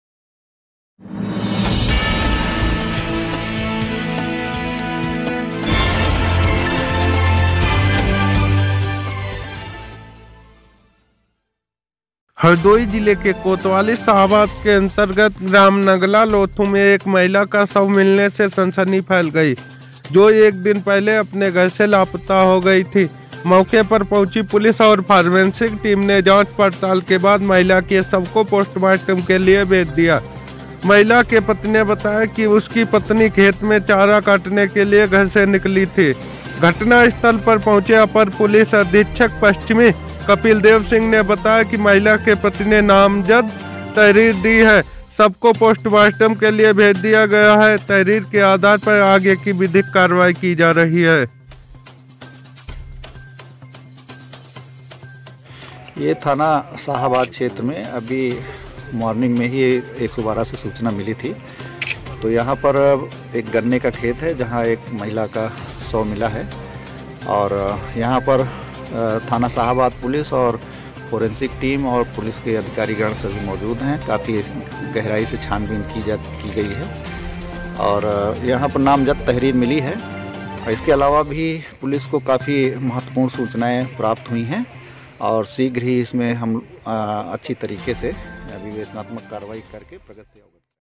संदिग्ध अवस्था में महिला का शव बरामद होने से फैली सनसनी
स्लग-संदिग्ध अवस्था में महिला का शव बरामद होने से फैली सनसनी

एंकर-हरदोई जिले के कोतवाली शाहाबाद के अंतर्गत ग्राम नगला लोथू में एक महिला का शव मिलने से सनसनी फैल गई जो एक  दिन पहले अपने घर से लापता हो गई थी मौके पर पहुंची पुलिस और फॉरेंसिक टीम ने जांच पड़ताल के बाद महिला के शव को पोस्टमार्टम के लिए भेज दिया है। महिला के पति ने बताया कि उसकी पत्नी खेत में चारा काटने के लिए घर से निकली थी।

विओ- घटनास्थल पर पहुंचे अपर पुलिस अधीक्षक पश्चिमी कपिल देव सिंह ने बताया है महिला के पति ने नामजद तहरीर दी है शव को पोस्टमार्टम के लिए भेज दिया गया है तहरीर के आधार पर आगे की कार्यवाही की जा रही है

 वाइट -अपर पुलिस अधीक्षक पश्चिमी कपिल देव सिंह